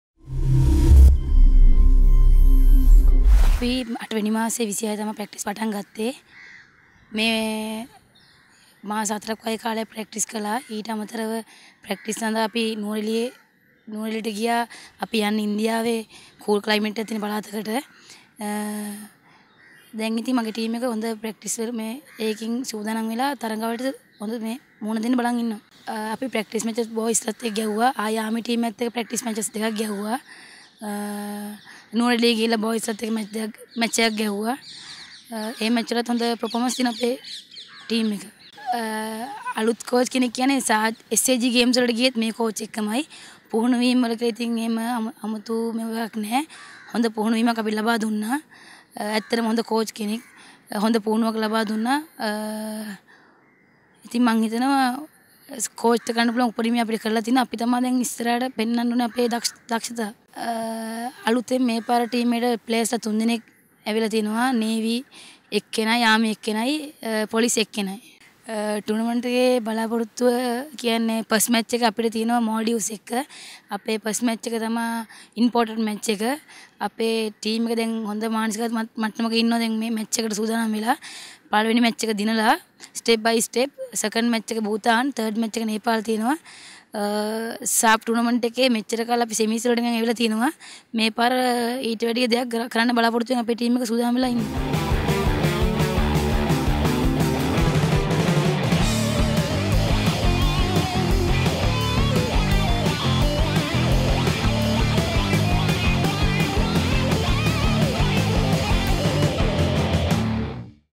Erandi Liyanage - Sri Lanka Women's Football Captain (SAFF Women's Championship)
Sri Lanka National Women's Football Team captain Erandi Liyanage shares her thoughts before leaving to India to participate in the 2016 SAFF Women's Championship.